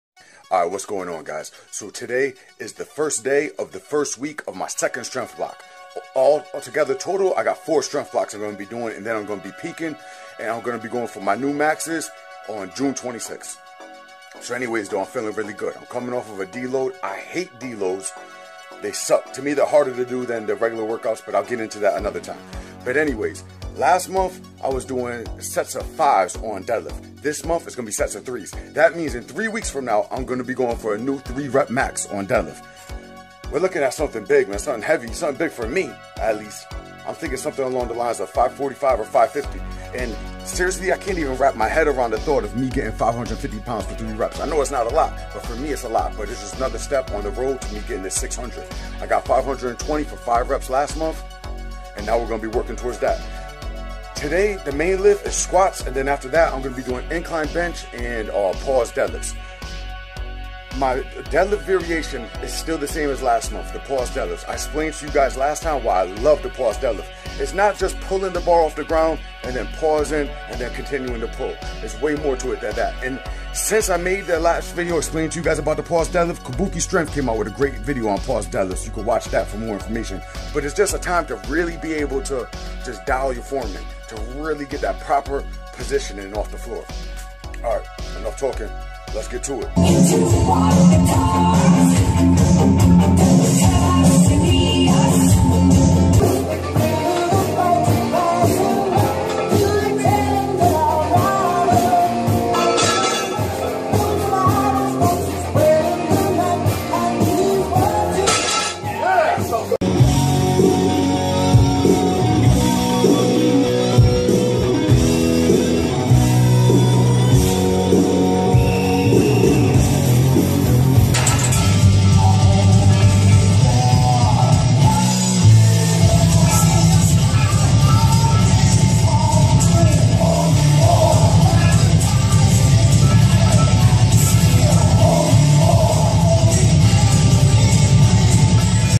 Building A 600lb Deadlift & 445lb Bench Press at 43 Years Old
today was the 1st day of my new strength block. I did 5x4 on squats. 4x2 on pause deadlifts. 4x4 on incline bench press. and 3x15 on SSB squats